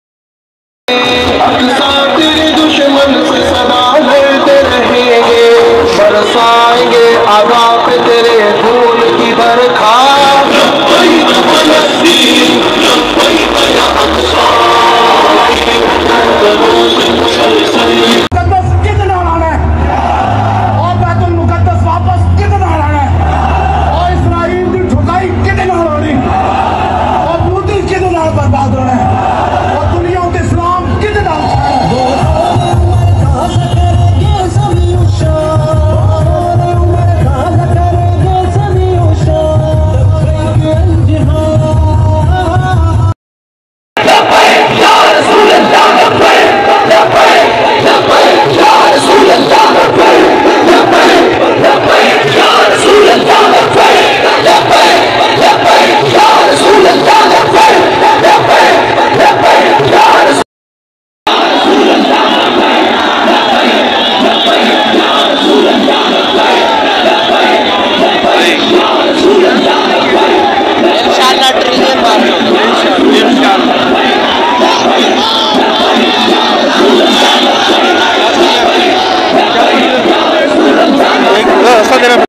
تحریک لبیک کا بہت بڑا جلوس
حضرت علامہ محمد سعد حسین رضوی صاحب
کہ انتظار میں اتنے سارے مخلوق کھڑی ہے